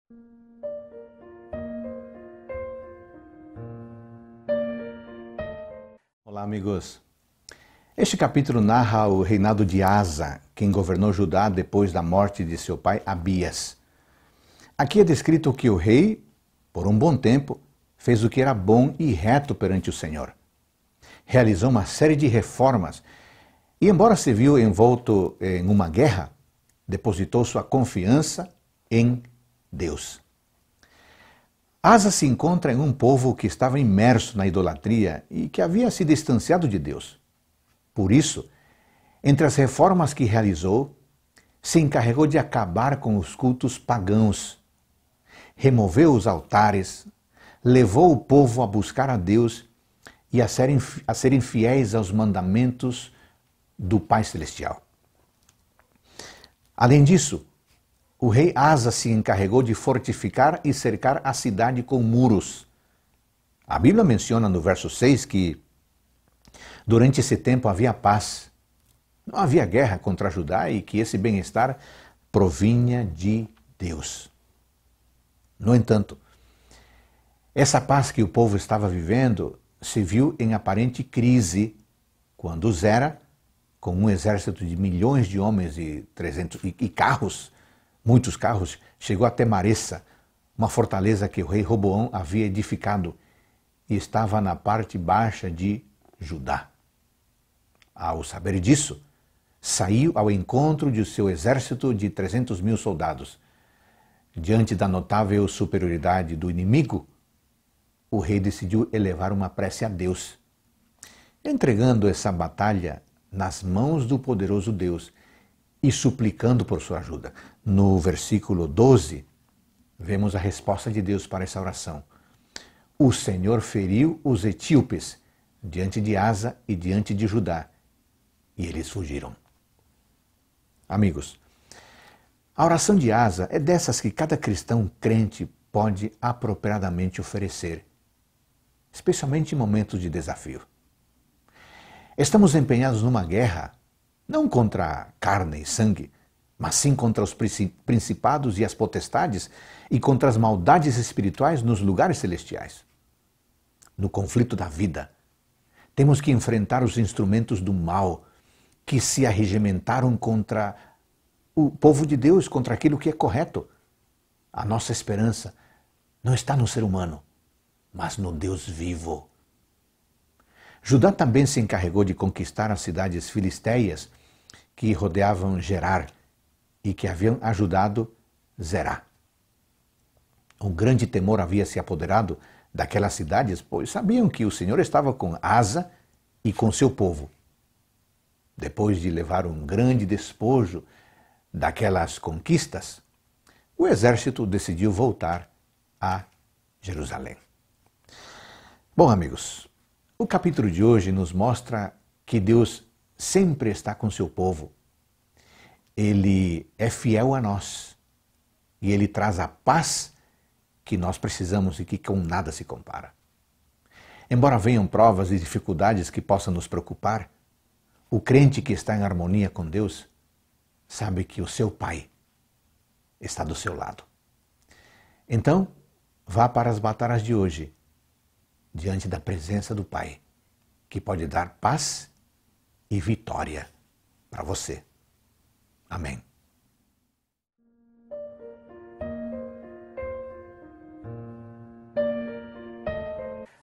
2 Crônicas 14 - Reavivados Por Sua Palavra
Ainda que venham provações e dificuldades que possam nos preocupar, o crente que está em harmonia com Deus sabe que está ao seu lado.